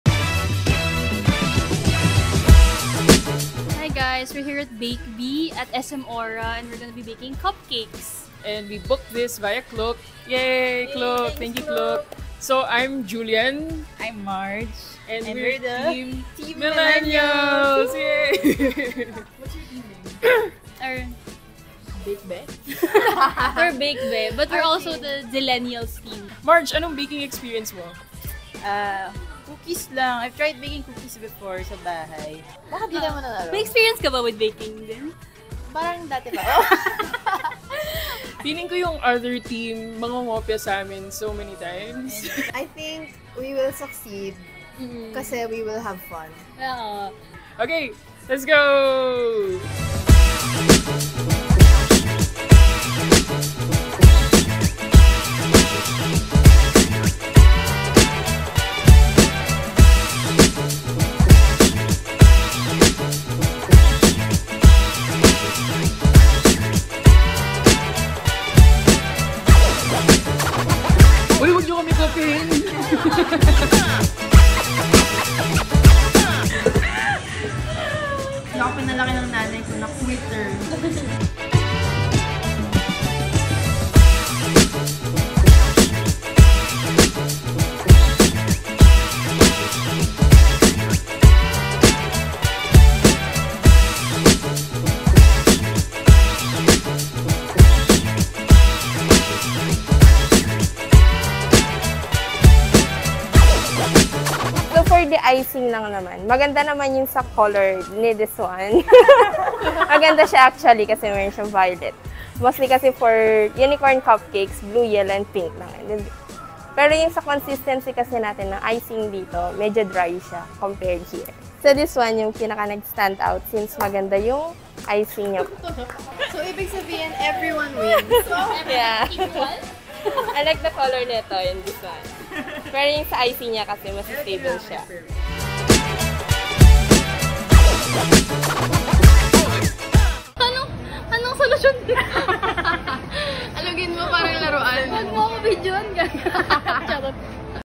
DIY unicorn cupcakes at BakeBe Studio
The BrandRap team does a bake-off! Watch who makes the better-looking unicorn cupcakes.